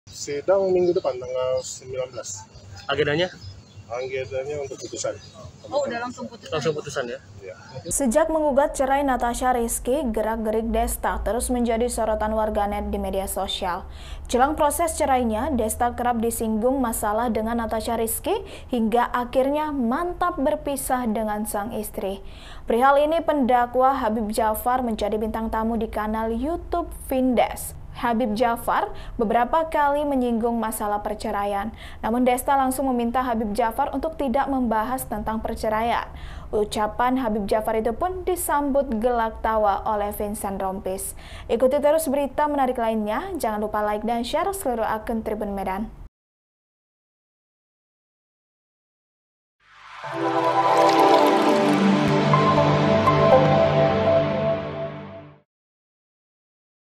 DESTA Skakmat Habib Jafar Gegara Bahas Perceraian, Reaksi Vincent Rompies Jadi Sorotan
Jelang proses cerainya, Desta kerap disinggung masalah dengan Natasha Rizky hingga akhinya mantap berpisah dengan sang istri.

Perihal ini, pendakwah Habib Jafar menjadi bintang tamu di kanal Youtube Vindes.

Banyak momen itu, Habib Jafar beberapa kali menyinggung masalah perceraian.